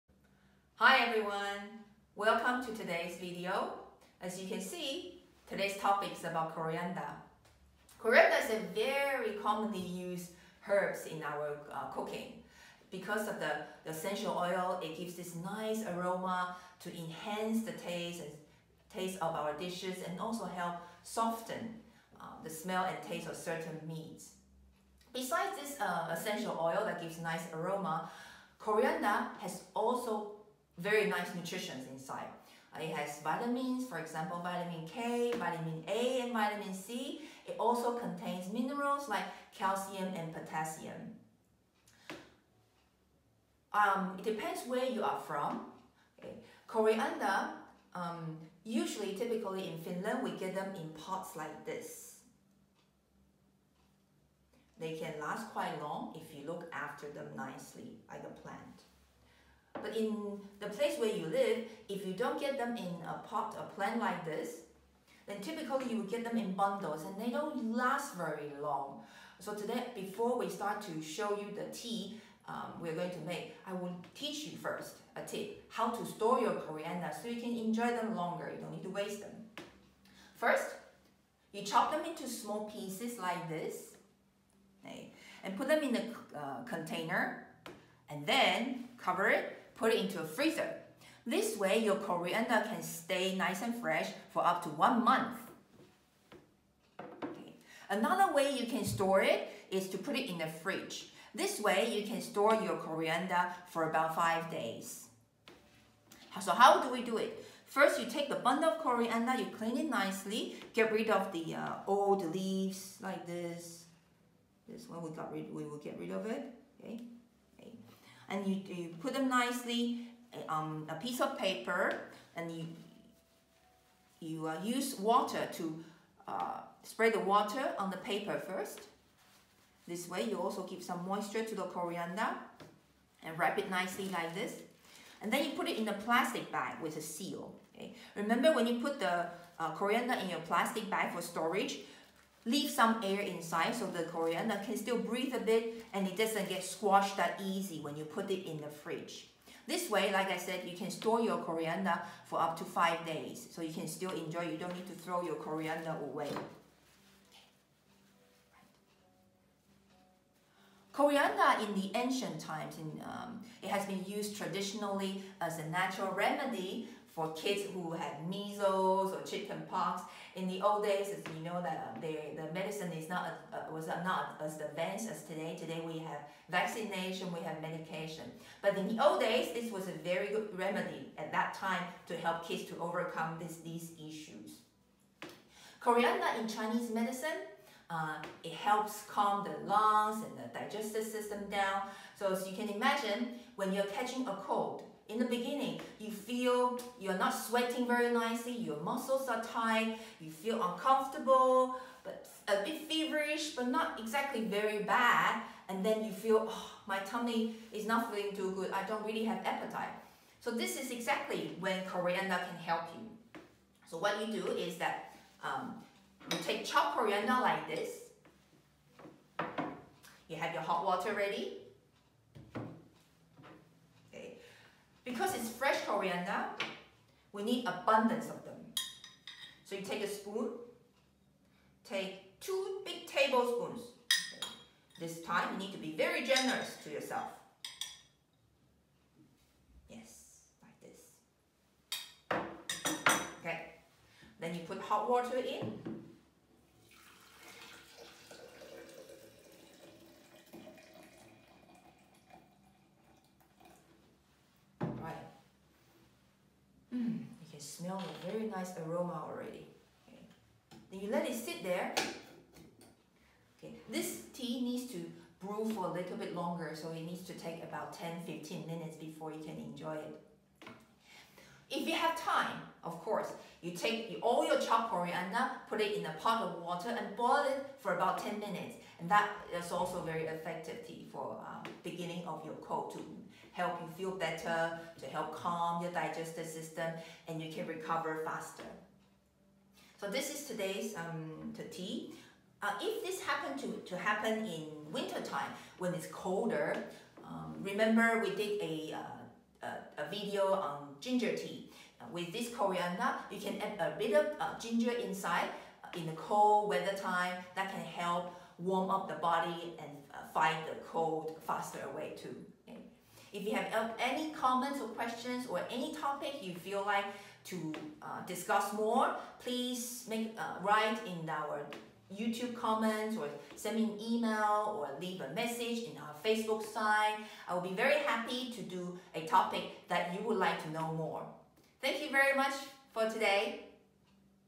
Coriander tea for early cold and flu symptoms - Tie Terveyteen akupunktio-klinikka
This video describes how to use coriander to help relieve the early symptoms of cold and flu.

Tie Terveyteen akupunktio-klinikka offers natural and effective solutions for pain, stress and many other health issues.